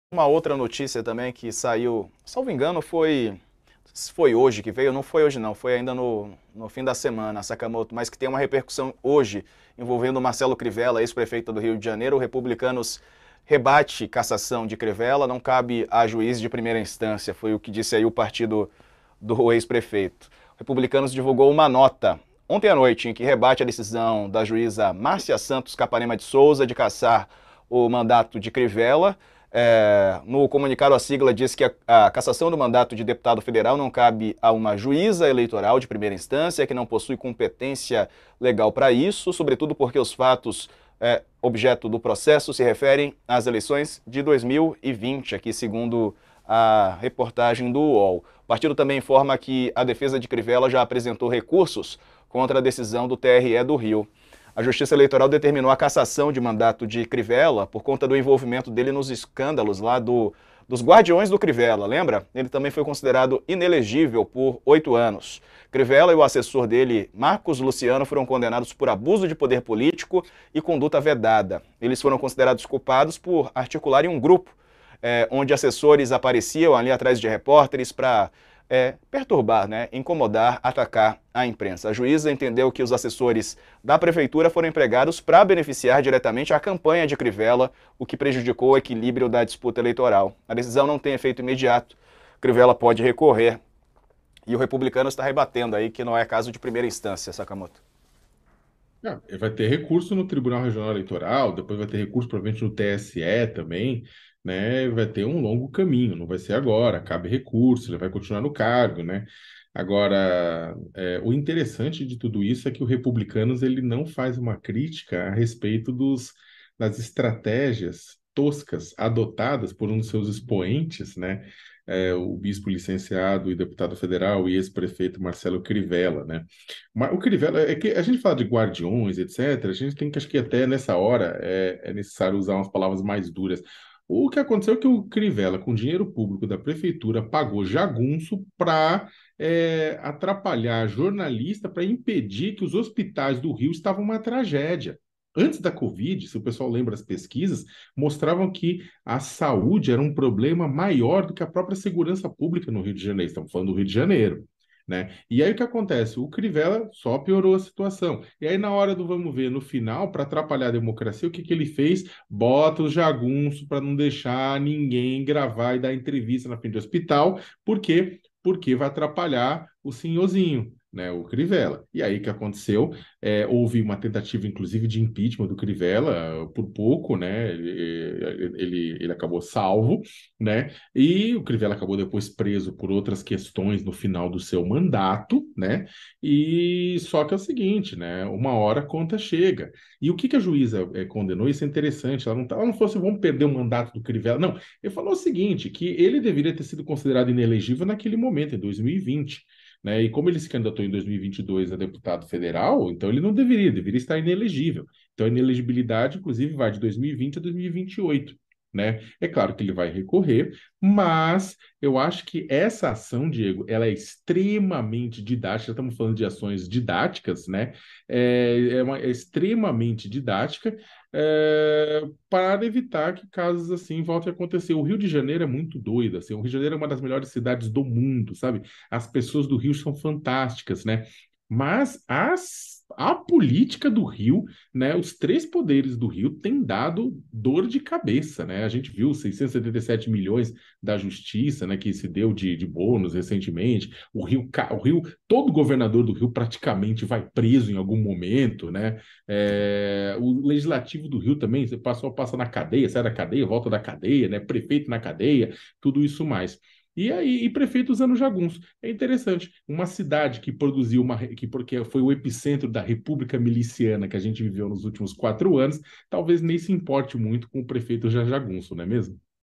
Crivella tem mandato cassado por bancar jagunços com grana pública no Rio | Leonardo Sakamoto
O colunista do UOL Leonardo Sakamoto comenta a cassação do mandato de deputado federal do ex-prefeito do Rio Marcelo Crivella.